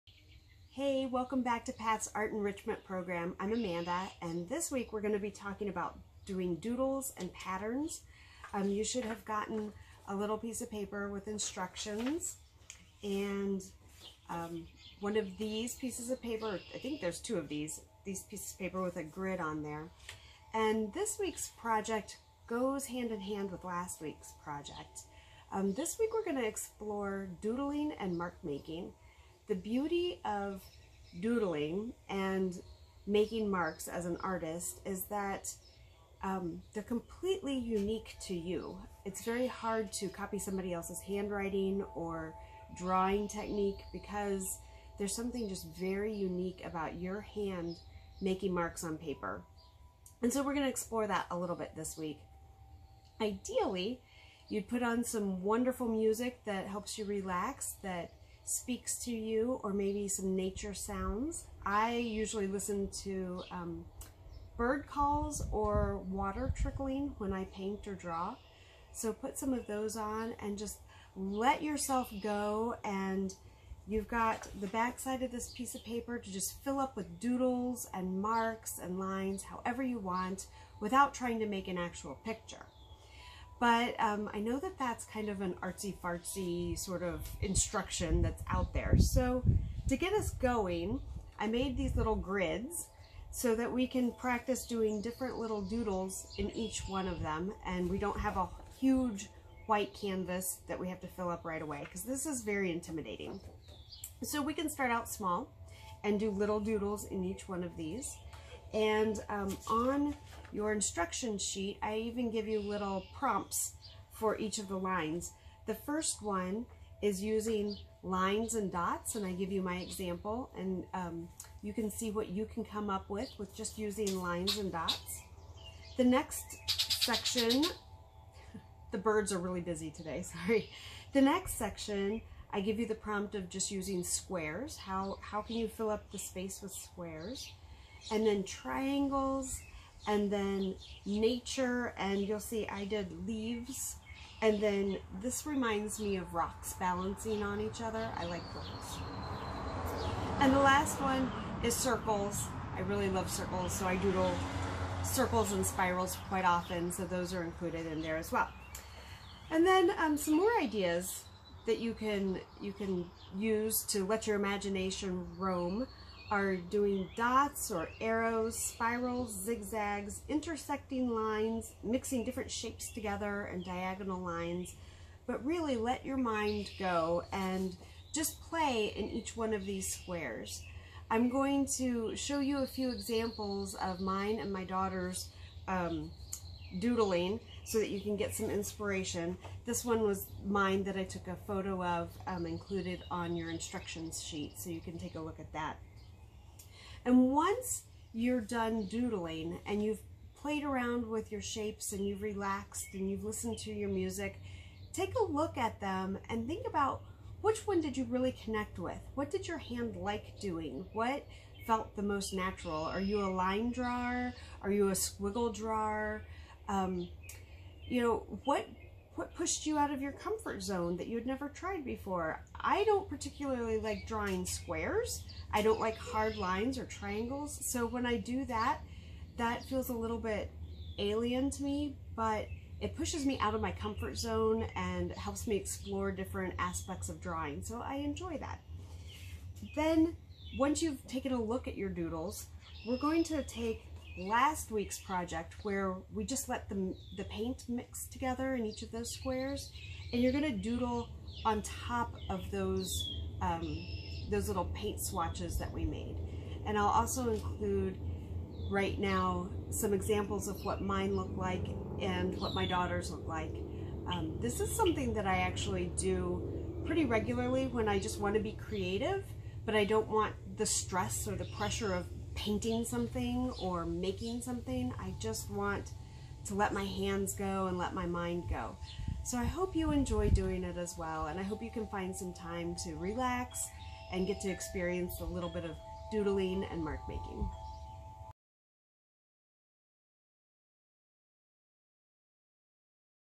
Mini Artworks 2: Doodling and Mark-Making
This week we'll use some prompts to set our imaginations free to doodle. Once you've played around with your doodles, you can create your final miniartwork by drawing over last week's watercolor project.